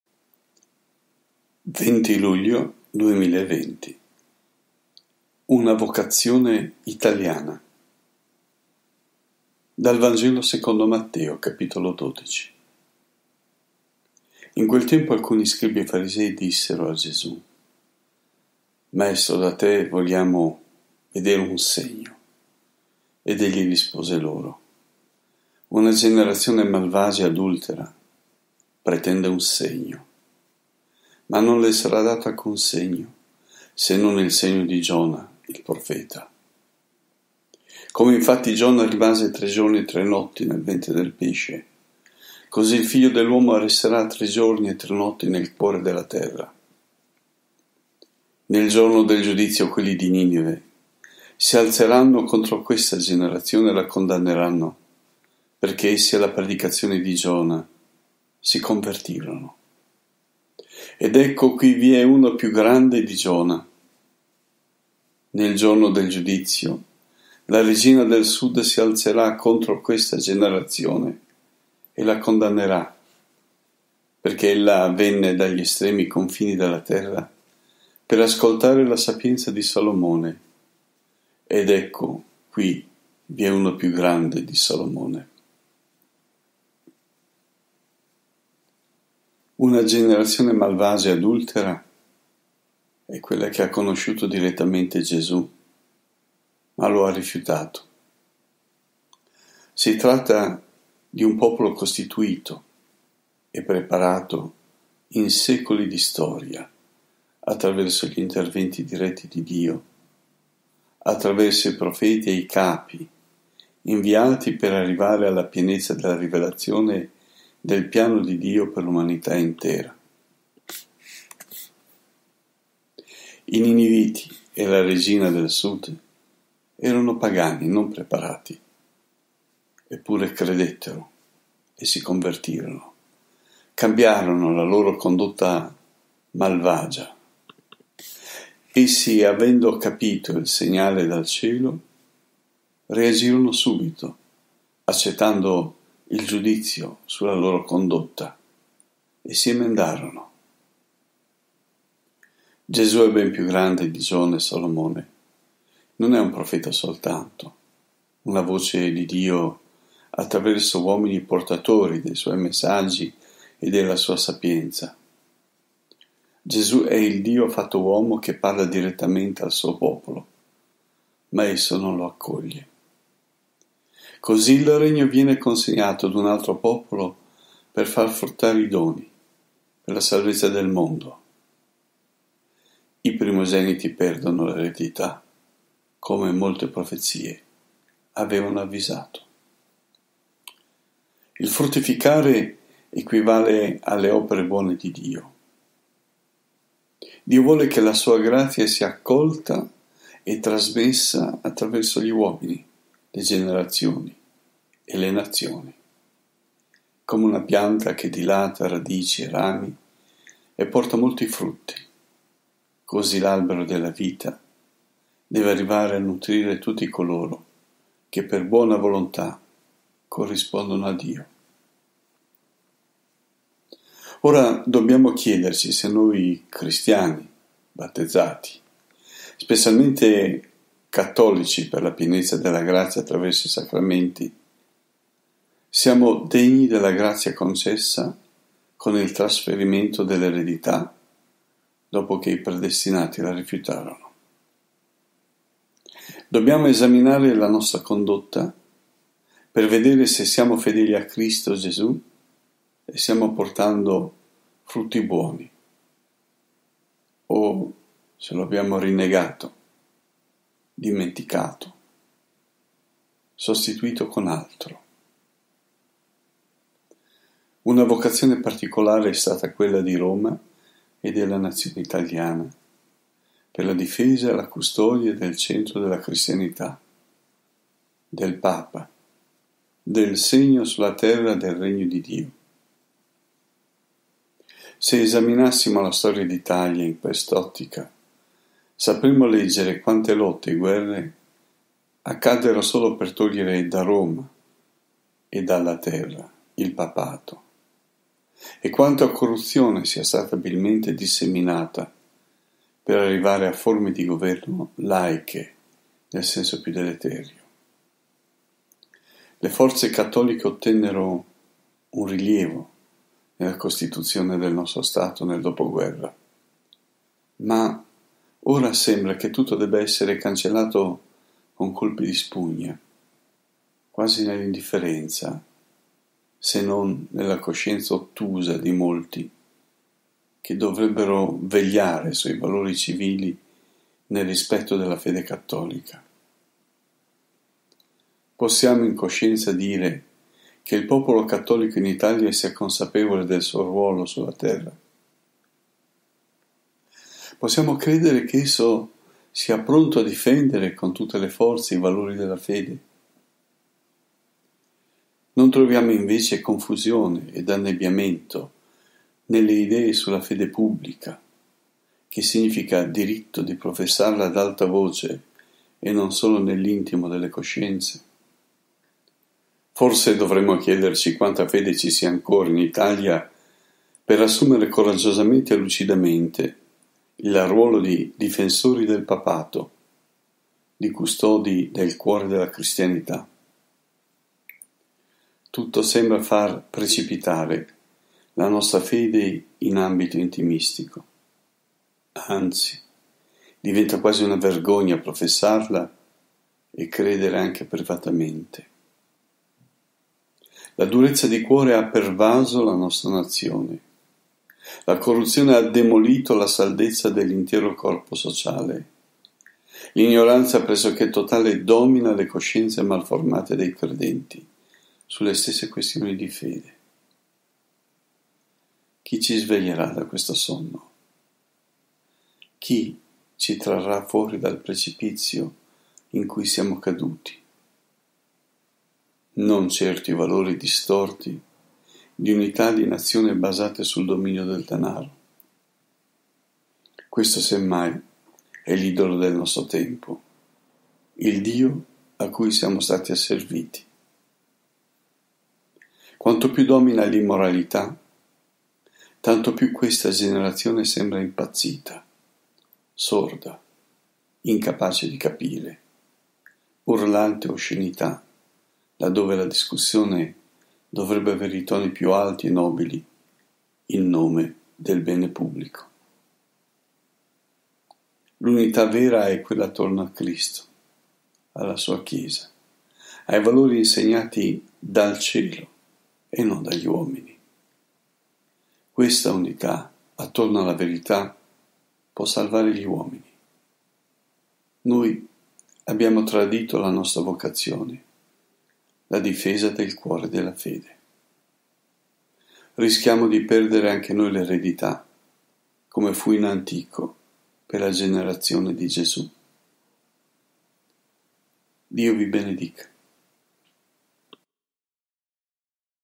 Una vocazione italiana - Commento al vangelo - 20.7.2020 - Mt 12,38-42
20 luglio 2020

UNA VOCAZIONE ITALIANA

Dal Vangelo secondo Matteo (Mt 12,38-42)
In quel tempo, alcuni scribi e farisei dissero a Gesù: «Maestro, da te vogliamo vedere un segno».
Ed egli rispose loro: «Una generazione malvagia e adultera pretende un segno! Ma non le sarà dato alcun segno, se non il segno di Giona il profeta. Come infatti Giona rimase tre giorni e tre notti nel ventre del pesce, così il Figlio dell’uomo resterà tre giorni e tre notti nel cuore della terra.
Nel giorno del giudizio, quelli di Nìnive si alzeranno contro questa generazione e la condanneranno, perché essi alla predicazione di Giona si convertirono. Ed ecco, qui vi è uno più grande di Giona! Nel giorno del giudizio, la regina del Sud si alzerà contro questa generazione e la condannerà, perché ella venne dagli estremi confini della terra per ascoltare la sapienza di Salomone. Ed ecco, qui vi è uno più grande di Salomone!».

La generazione malvagia e adultera è quella che ha conosciuto direttamente Gesù, ma lo ha rifiutato. Si tratta di un popolo costituito e preparato in secoli di storia, attraverso gli interventi diretti di Dio attraverso i profeti e i capi inviati per arrivare alla pienezza della rivelazione del piano di Dio per l’umanità intera.
I Niniviti e la Regina del Sud erano pagani non preparati, eppure credettero e si convertirono, cambiarono la loro condotta malvagia. Essi, avendo capito il segnale dal cielo, reagirono subito, accettando il giudizio sulla loro condotta e si emendarono.
Gesù è ben più grande di Giona e Salomone, non è un profeta soltanto, una voce di Dio attraverso uomini portatori dei suoi messaggi e della sua sapienza: Gesù è il Dio fatto uomo che parla direttamente al suo popolo, ma esso non lo accoglie.
Così il Regno viene consegnato ad un altro popolo per far fruttare i doni, per la salvezza del mondo. I primogeniti perdono l’eredità, come molte profezie avevano avvisato.
Il fruttificare equivale alle opere buone di Dio. Dio vuole che la sua grazia sia accolta e trasmessa attraverso gli uomini, le generazioni e le nazioni. Come una pianta che dilata radici e rami e porta molti frutti, così l’albero della vita deve arrivare a nutrire tutti coloro, che per buona volontà corrispondono a Dio.
Ora dobbiamo chiederci se noi Cristiani, battezzati, specialmente cattolici per la pienezza della grazia attraverso i sacramenti, siamo degni della grazia concessa con il trasferimento dell’eredità, dopo che i predestinati la rifiutarono. Dobbiamo esaminare la nostra condotta, per vedere se siamo fedeli a Cristo Gesù e stiamo portando frutti buoni o se lo abbiamo rinnegato, dimenticato, sostituito con altro.
Una vocazione particolare è stata quella di Roma e della nazione italiana per la difesa e la custodia del centro della cristianità, del Papa, del segno sulla terra del regno di Dio.
Se esaminassimo la Storia d’Italia in quest’ottica sapremmo leggere quante lotte e guerre accaddero solo per togliere da Roma e dalla terra il papato, e quanta corruzione sia stata abilmente disseminata per arrivare a forme di governo laiche, nel senso più deleterio.
Le forze cattoliche ottennero un rilievo nella Costituzione del nostro Stato nel dopoguerra, ma ora sembra che tutto debba essere cancellato con colpi di spugna, quasi nell’indifferenza se non nella coscienza ottusa di molti, che dovrebbero vegliare sui valori civili nel rispetto della fede cattolica.
Possiamo in coscienza dire che il popolo cattolico in Italia sia consapevole del suo ruolo sulla terra? Possiamo credere che esso sia pronto a difendere con tutte le forze i valori della fede? Non troviamo invece confusione ed annebbiamento nelle idee sulla fede pubblica, che significa diritto di professarla ad alta voce e non solo nell’intimo delle coscienze?
Forse dovremmo chiederci quanta fede ci sia ancora in Italia per assumere coraggiosamente e lucidamente, il ruolo di difensori del papato, di custodi del cuore della cristianità.
Tutto sembra far precipitare la nostra fede in ambito intimistico, anzi, diventa quasi una vergogna professarla e credere anche privatamente. La durezza di cuore ha pervaso la nostra nazione, la corruzione ha demolito la saldezza dell’intero corpo sociale, l’ignoranza pressoché totale domina le coscienze malformate dei credenti, sulle stesse questioni di fede.
Chi ci sveglierà da questo sonno? Chi ci trarrà fuori dal precipizio in cui siamo caduti? Non certo i valori distorti di unità di nazioni basate sul dominio del denaro, questo semmai è l’idolo del nostro tempo, il dio a cui siamo stati asserviti.
Quanto più domina l’immoralità, tanto più questa generazione sembra impazzita, sorda, incapace di capire, urlante oscenità là dove la discussione dovrebbe avere i toni più alti e nobili in nome del bene pubblico.
L’unità vera è quella attorno a Cristo, alla sua Chiesa, ai valori in